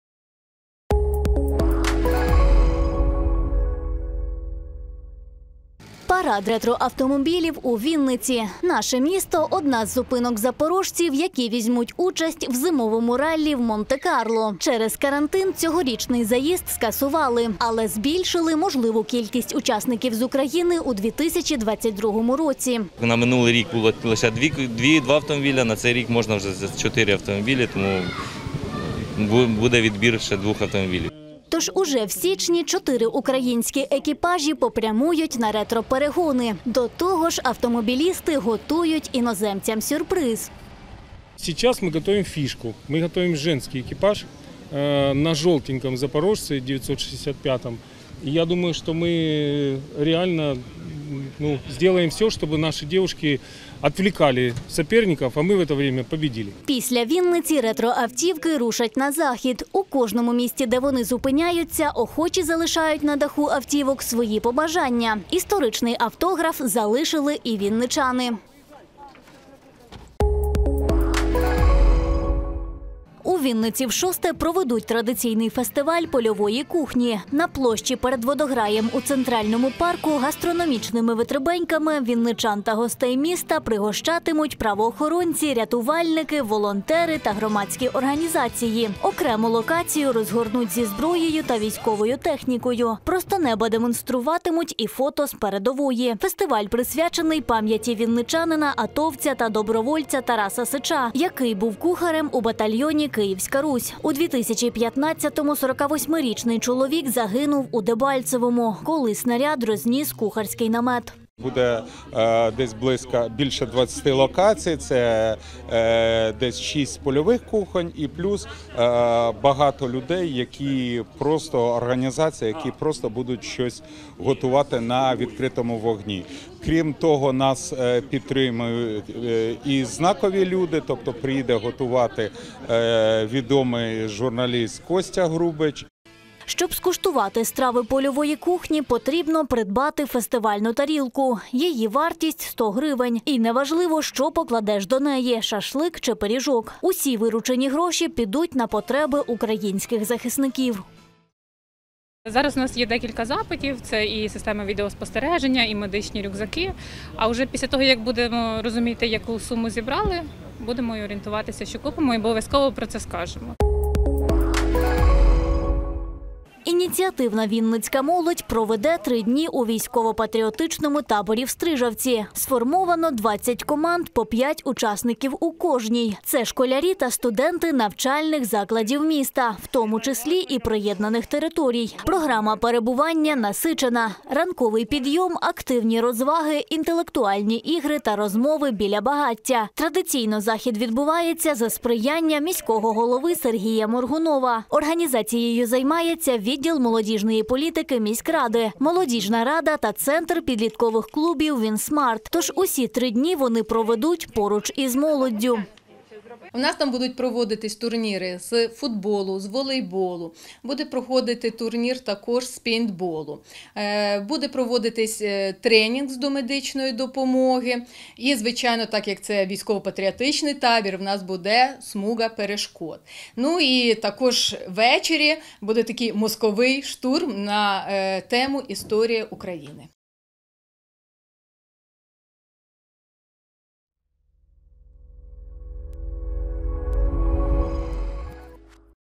Важливо 2021-06-23 16:00
Важливо: Дайджест актуальних новин за 23 червня 2021 року

Тема випуску:
- Парад ретро-автомобілів у Вінниці
- Вінничан запрошують на VI фестиваль польової кухні пам'яті Тараса Сича
- Ініціативна вінницька молодь проведе три дні у військово-патріотичному таборі